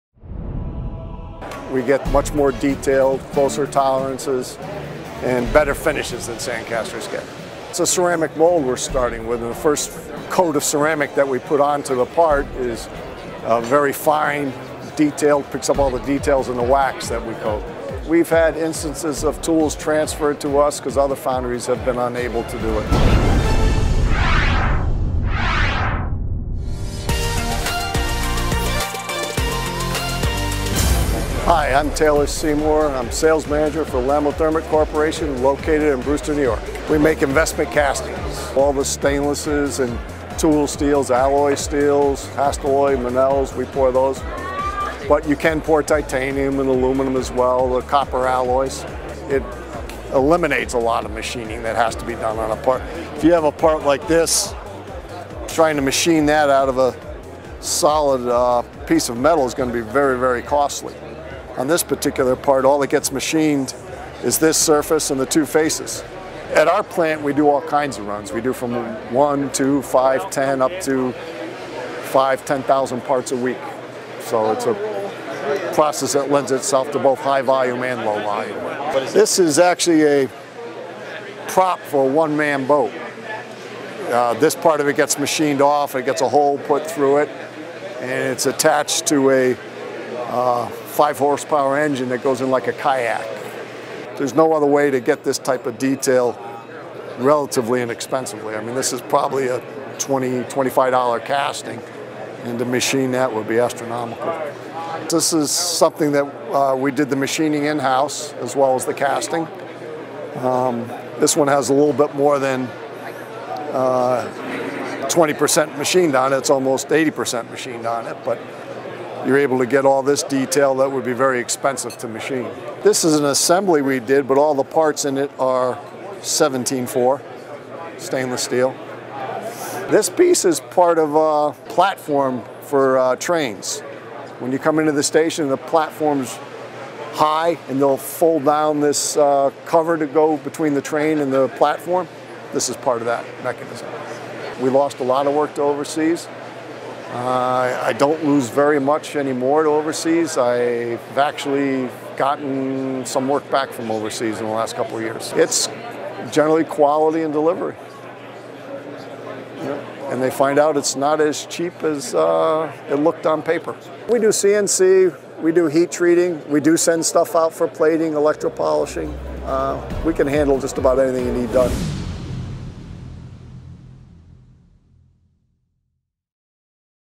Stainless Steel Precision Investment Casting | Lamothermic | Brewster, NY
LAMOTHERMIC - Stainless Steel Precision Investment Casting

Lamothermic is your premier supplier of precision investment castings – quality first, every time. Our commitment to superior quality combined with our best in class engineering and advanced automation means that our customers get the best castings for their needs.

 Lamothermic wants to partner with you for all your investment casting needs. Whether your part needs are the size of a penny or a tire, stainless steel or bronze, you can count on Lamothermic to deliver top quality castings. We service a broad base of industries, including aerospace, construction, transportation, energy, defense and a wide variety of commercial applications.

 Lamothermic provides products and services that consistently meet our customer specified requirements. We are committed to achieving customer satisfaction through excellent customer service, workmanship, teamwork and continuous improvement to our manufacturing and the quality management systems.

One of our capabilities is spectrographic analysis.

Our spectrometer is used to monitor and verify compliance to chemical requirements of incoming metals and work in process. We also do non-destructive inspections, both radiographic and X-ray. Our Nondestructive Testing (NDT) capabilities include Radiography, Liquid Penetrant, and Magnetic Particle. Lamothermic NDT Inspectors are qualified to the guidelines of the American Society for Nondestructive Testing Recommended Practice No. SNT-TC-1A and National Aerospace Standard NAS-410. Radiographic, or X-Ray non-destructive testing is used to reveal internal, surface and sub-surface irregularities in many industrial castings and provides a permanent record. Liquid penetrant Inspection is sensitive in locating surface discontinuities used on ferrous and nonferrous alloys. Castings are coated with a penetrating liquid which is drawn into defective areas and is then detected using UV lighting. Magnetic Particle Inspection, or MPI is for use on ferrous materials, which can detect minute surface cracks and discontinuities that are open to the surface.

 Process control is an integral part of our quality management system with technical assistance from our engineering staff. We have laboratory control of our incoming wax, ceramic shell slurrys and for controlling and checking our alloy chemistries.